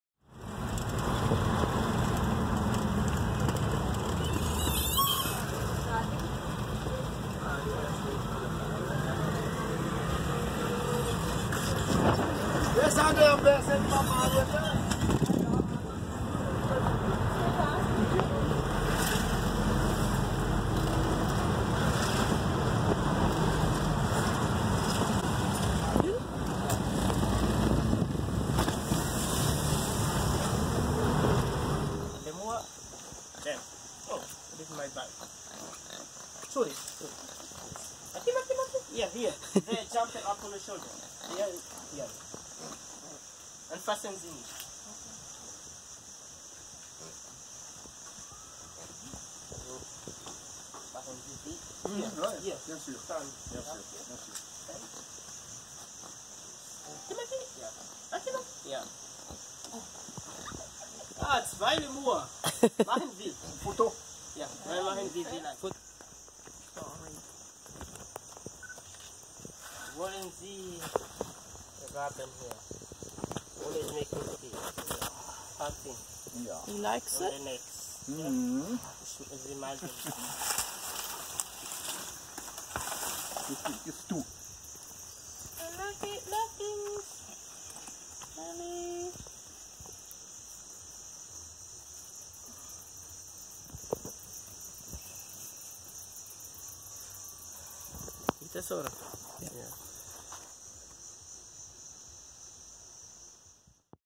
Visit to Lemuria Park, Nosy Be
Visit to Lemuria Park, Nosy Be, Madagascar. Petting Lemurs and Turtles!